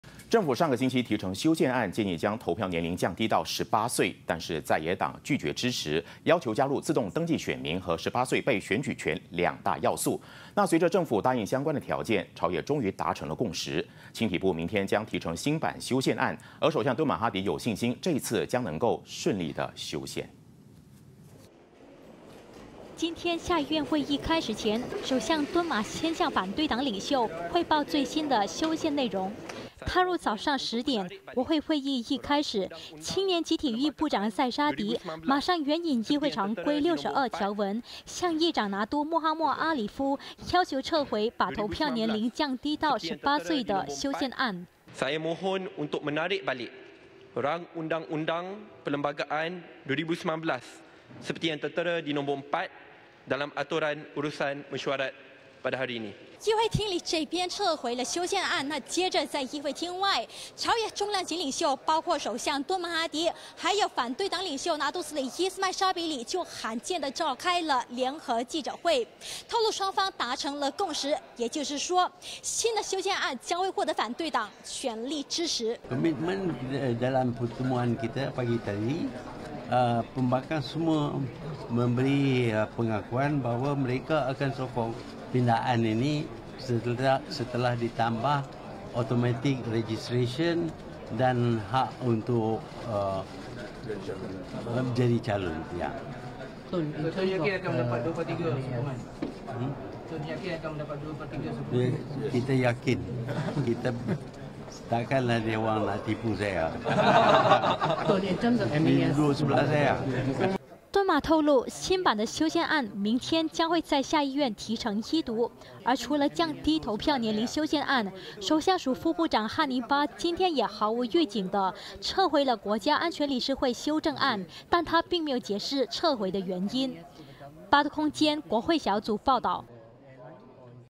【政府同意在野党二条件 投票年龄修宪案有望过关】
政府上个星期提呈修宪案，建议将投票年龄降低到18岁，但在野党拒绝支持，要求加入自动登记选民和18岁被选举权两大要素。随着政府答应相关条件，朝野终于达成共识。青体部明天将提呈新版修宪案，而首相敦马哈迪有信心这次将能顺利修宪。